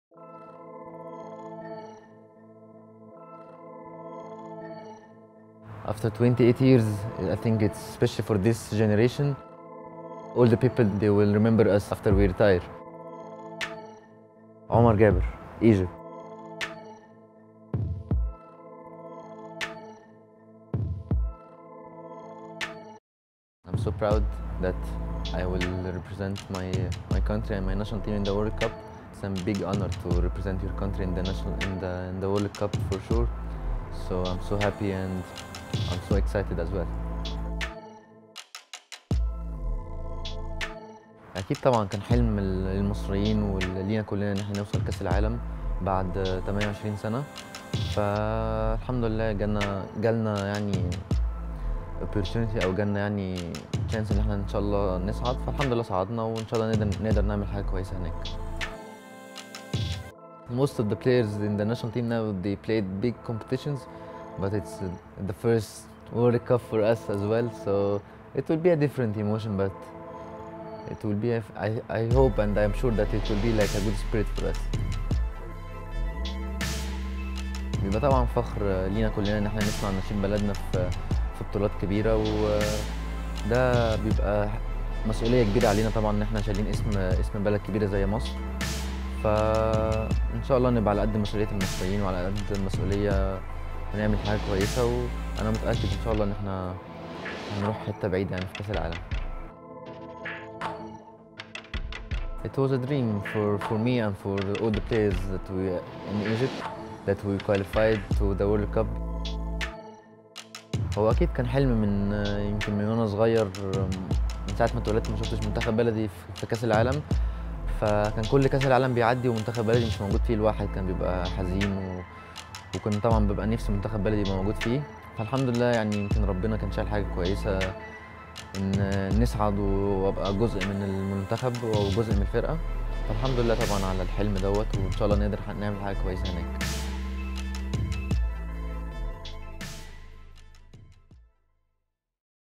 For My Country: Omar Gaber and Egypt at FIFA World Cup
Omar Gaber and Egypt begin their World Cup run on Friday, June 15. The last time Egypt qualified for the World Cup was 28 years ago in 1990, before Omar was born.